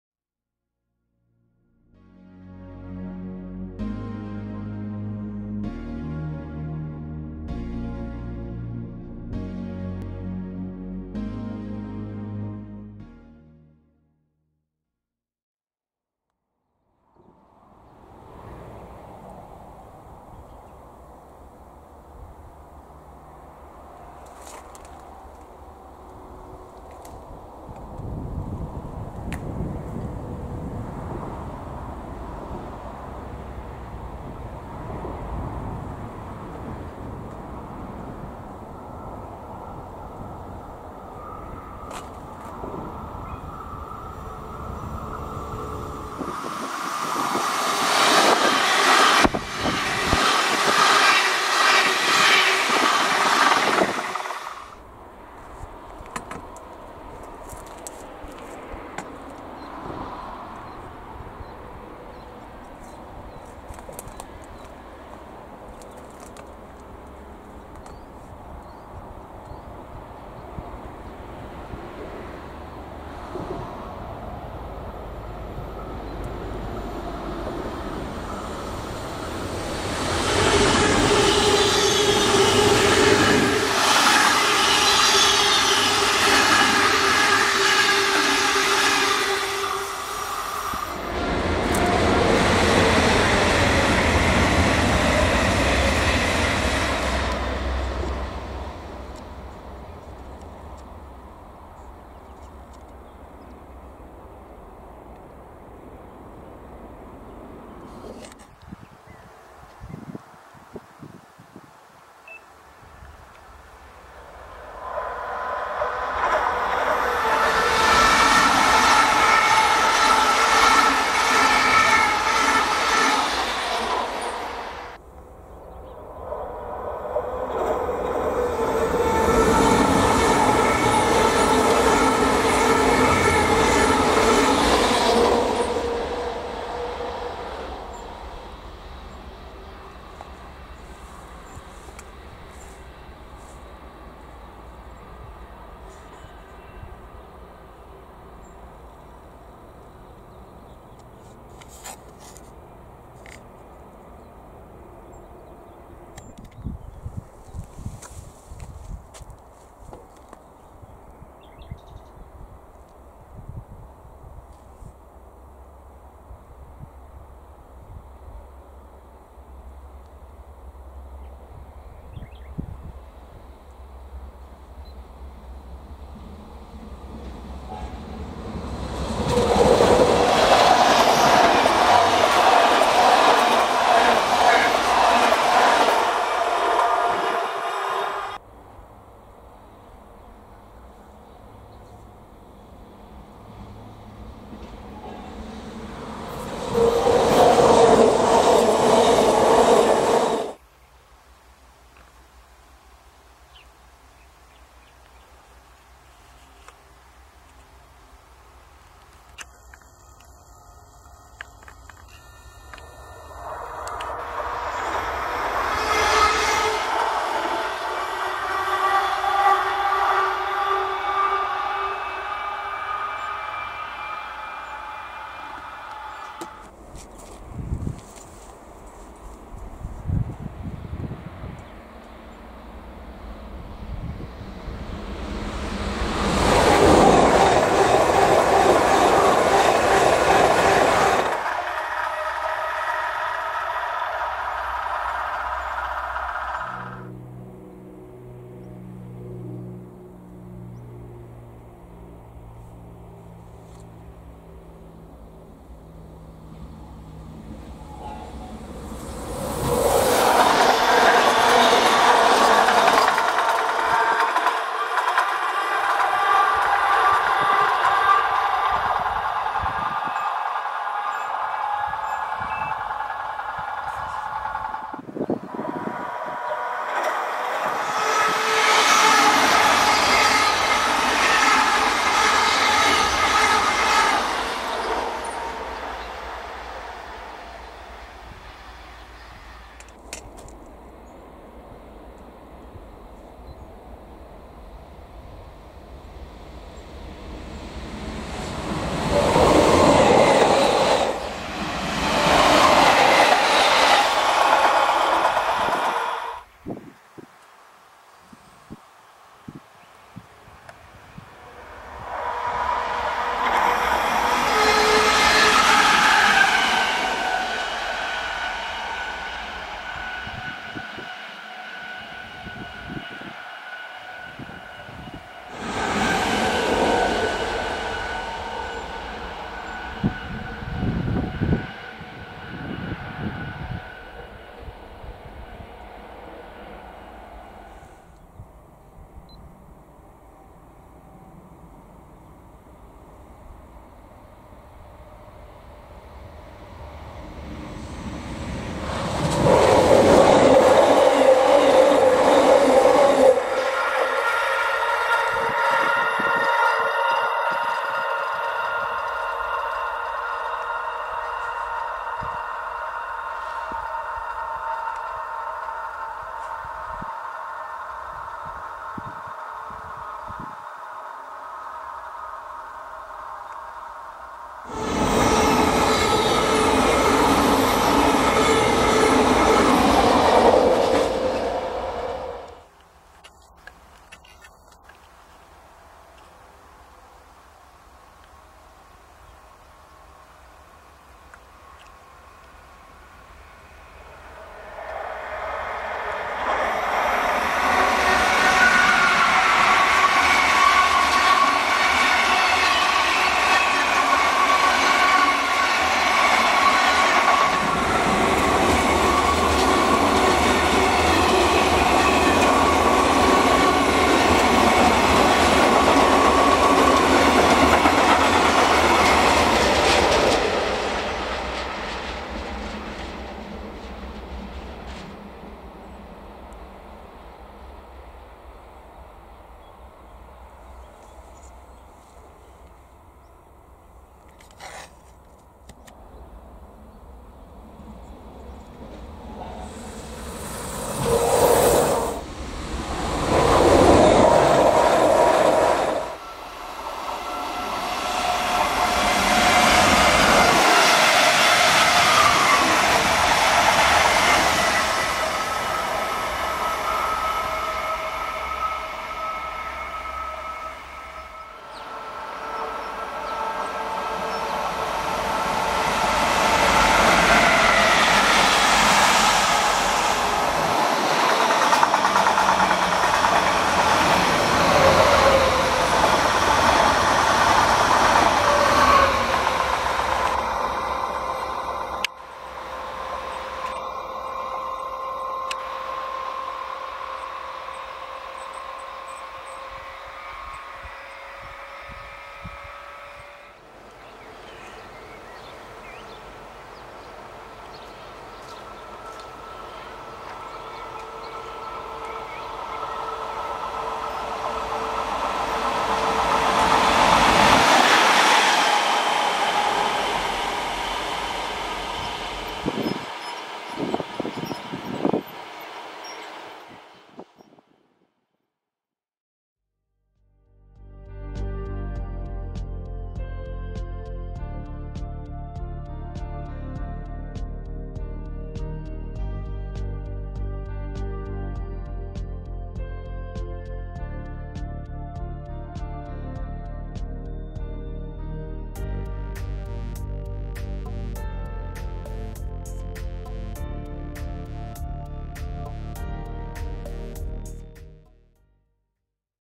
Trainspotting Clip 19 - die Aufnahmen entstanden zwischen Schifferstadt und Böhl-Iggelheim
Aufnahmen, die mir gefallen ... und vielleicht auch euch.

Der Bahnverkehr an der Strecke zwischen Schifferstadt und Böhl-Iggelheim. Standort war an der Abzweigung der "Ortsumgehung" Schifferstadt.
Natürlich waren auch hier wieder viele S-Bahnen unterwegs. Sie halten halt den VRN hoch!!!